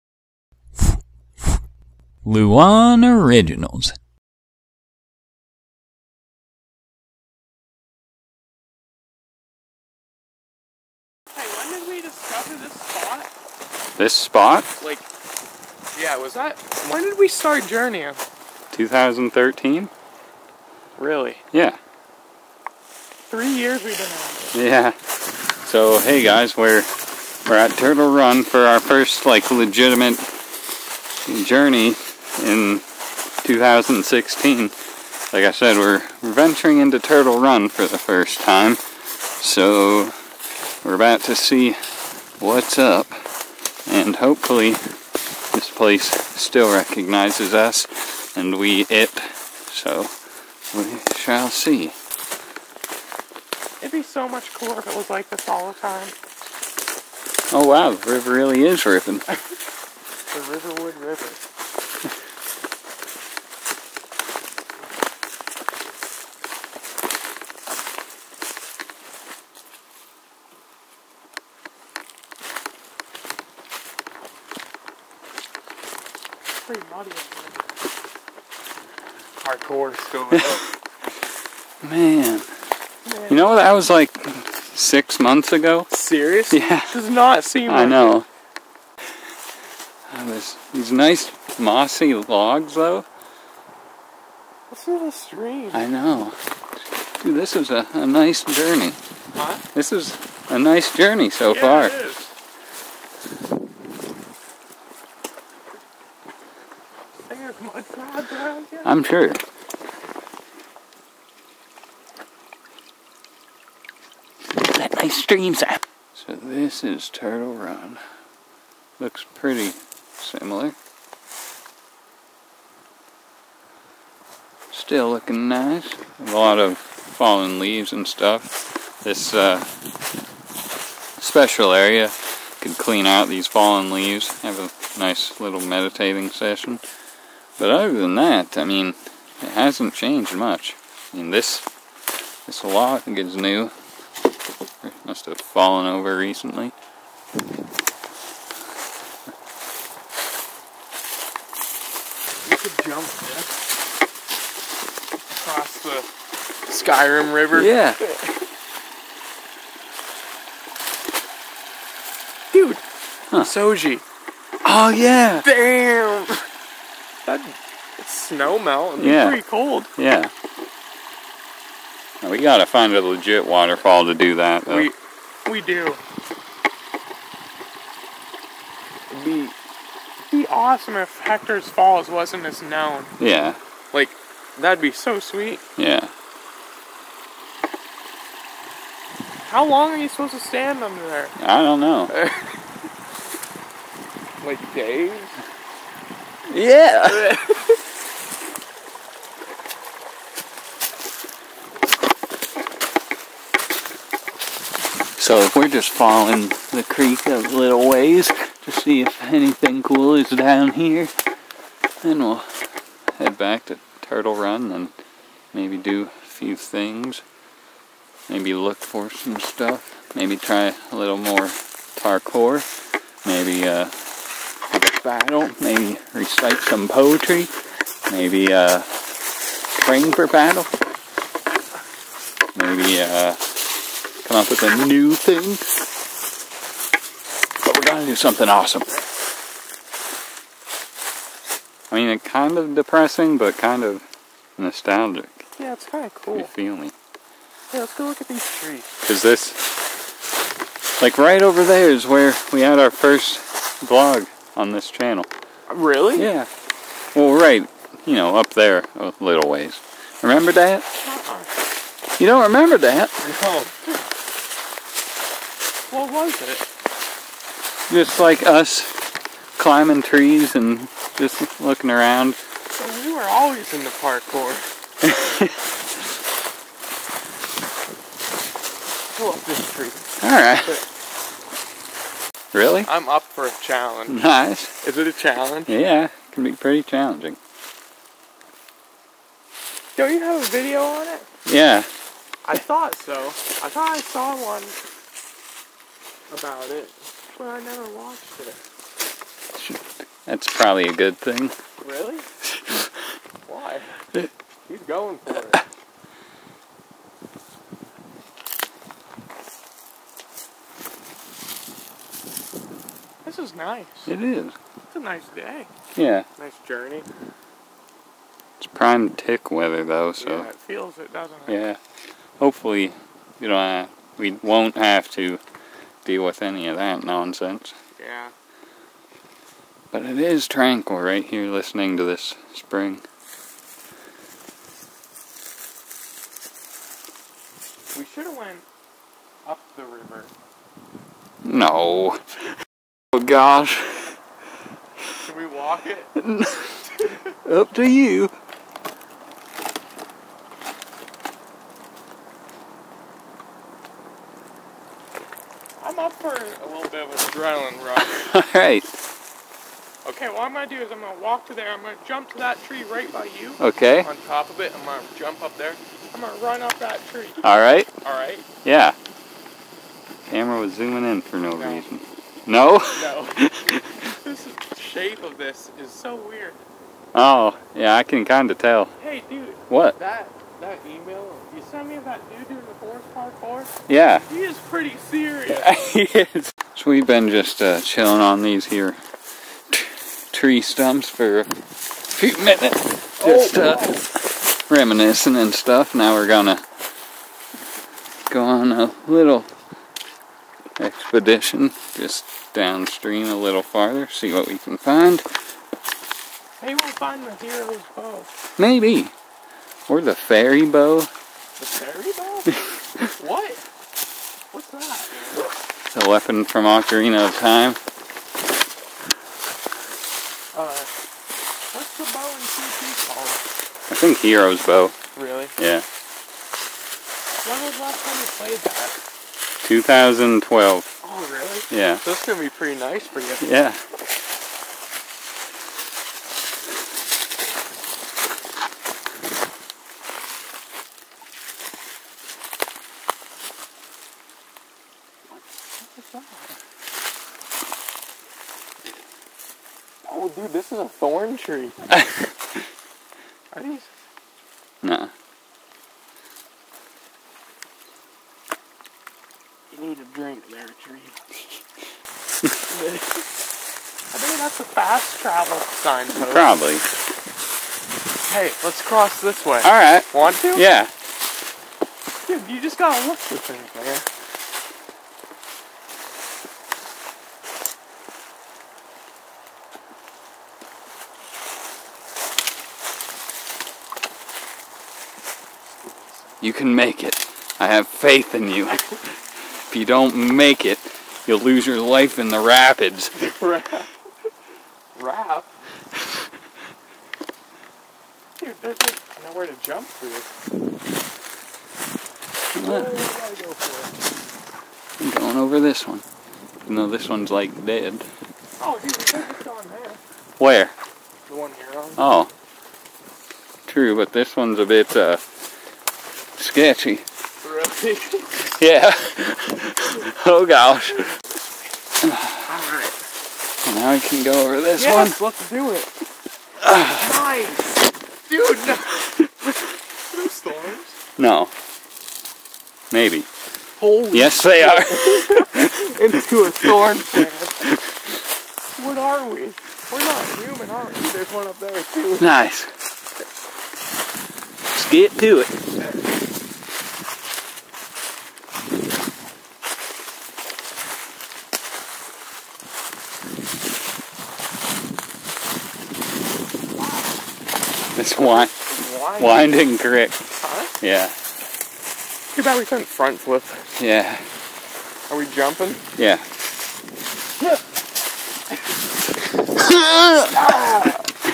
VLOG Day 2 Part 1 - Nice Spring Day At Turtle Run
Lukas and Andy spend the day at Turtle Run and explore the surrounding areas during early spring 2016. The scenery was not only very calm and pretty, but also heavily nostalgic for some reason.
All around, a very enjoyable day.